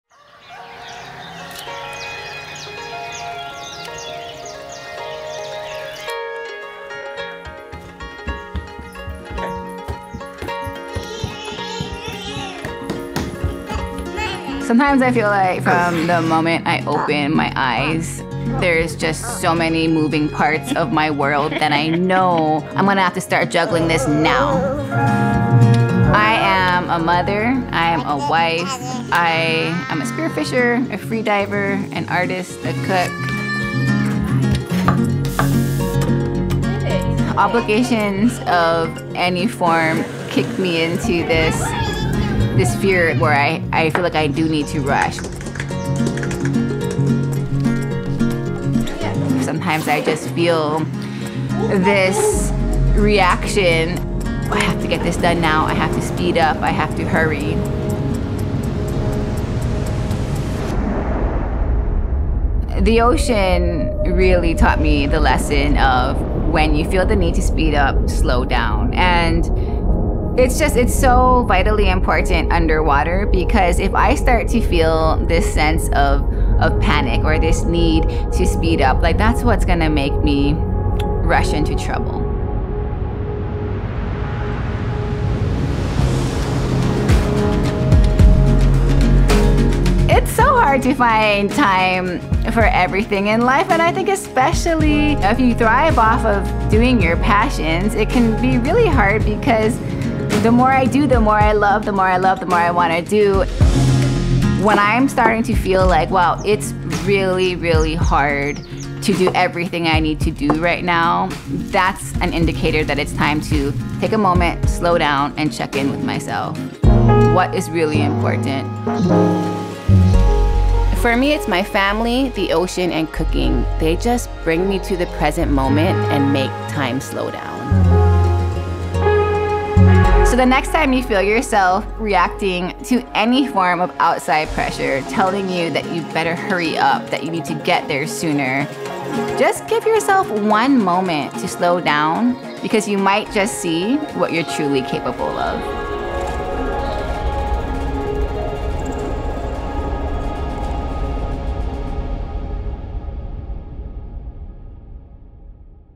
SLOW DOWN: Kimi Werner x MVMT
As a champion free diver, spear fisher and Hawaiian native, Kimi’s life is deeply tied to the wonder and well-being of our oceans. We spent time in her home community of Waialua, Hawaii, where she took our Ocean Plastic Sport where it’s meant to be: under the water, on island time.